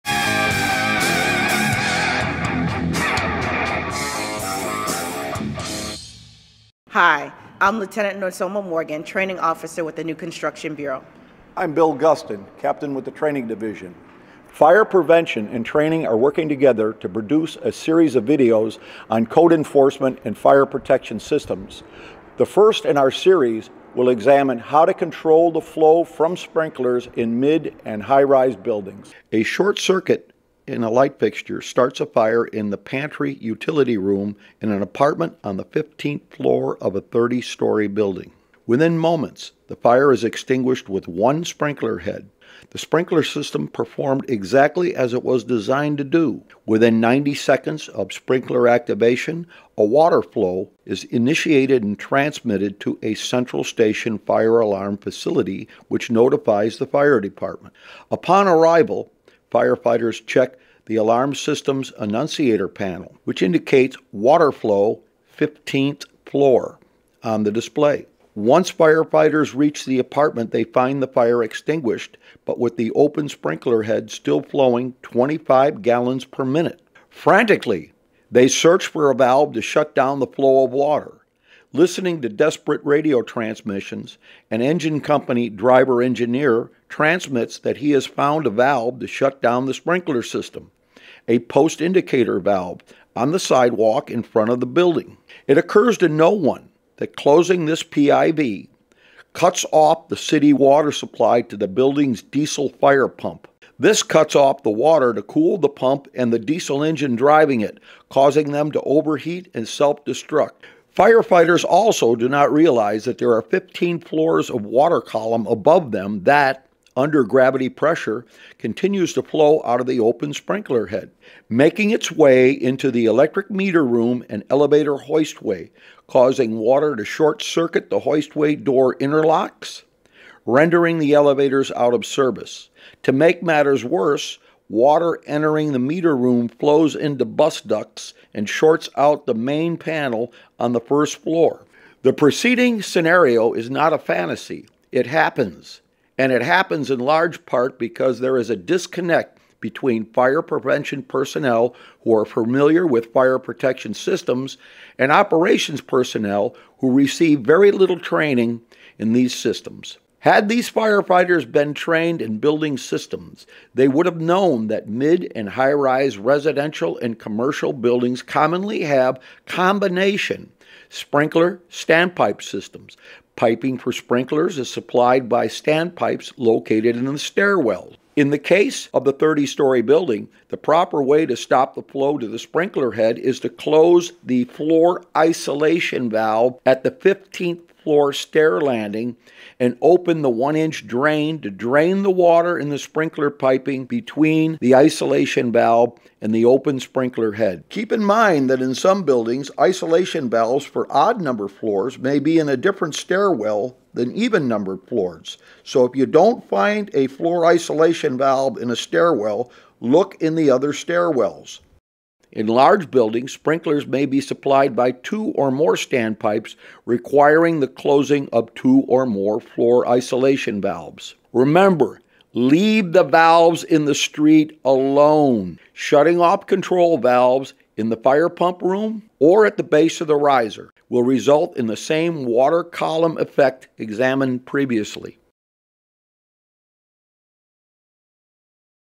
How to Control Sprinkler Flow in Mid- and High-Rise Buildings
Miami-Dade (FL) Fire Lieutenant Nassoma Morgan and Captain Bill Gustin offer sprinkler flow training.

Courtesy of @MiamiDadeFireRescue